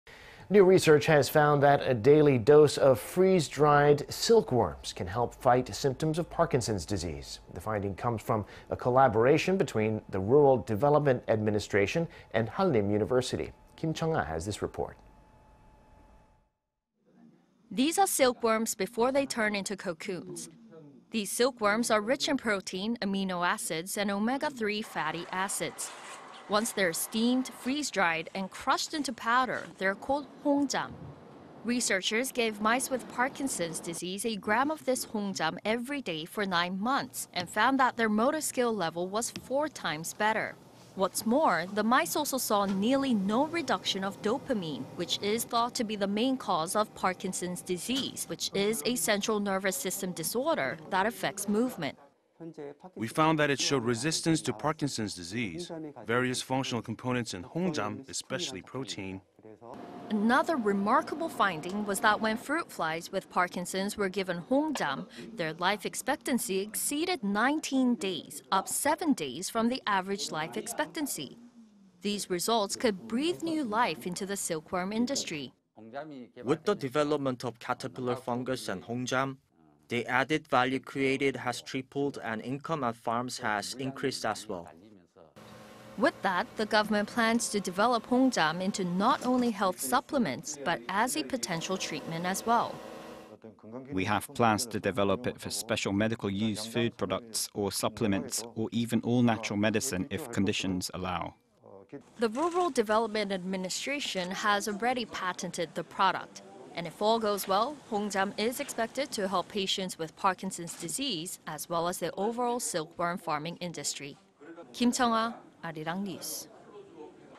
Silkworms found to be effective against Parkinson's disease
동결건조한 누에 '홍잠'…파킨슨병 증상 억제 효과

New research has found that a daily dose of freeze-dried silkworms can help fight symptoms of Parkinson's disease.
The finding comes from a collaboration between the Rural Development Administration and Hallym   University.
Kim Cheong-ah has this report.
These are silkworms before they turn into cocoons.
These silkworms are rich in protein, amino acids and omega-3 fatty acids.
Once they are steamed, freeze dried and crushed into powder, they are called "hongjam."
Researchers gave mice with Parkinson's disease a gram of this hongjam every day for 9 months, and found that their motor skill level was four times better.
What's more, the mice also saw nearly no reduction of dopamine, which is thought to be the main cause of Parkinson's disease... which is a central nervous system disorder that affects movement.
" We found that it showed resistance to Parkinson's disease. Various functional components in Hongjam, especially protein…"
Another remarkable finding was that when fruit flies with Parkinson's were given hongjam, their life expectancy exceeded 19 days, up 7 days from the average life expectancy.
These results could breathe new life into the silkworm industry.
"With the development of caterpillar fungus and Hongjam, the added value created has tripled and income at farms has increased as well."
With that, the government plans to develop Hongjam into not only health supplements but as a potential treatment as well.
"We have plans to develop it for special medical-use food products or supplements or even all-natural medicine if conditions allow…"
The rural development administration has already patented the product...and if all goes well,  Hongjam is expected to help patients with Parkinson's disease as well as the overall silkworm farming industry.
Kim Cheong-ah, Arirang News.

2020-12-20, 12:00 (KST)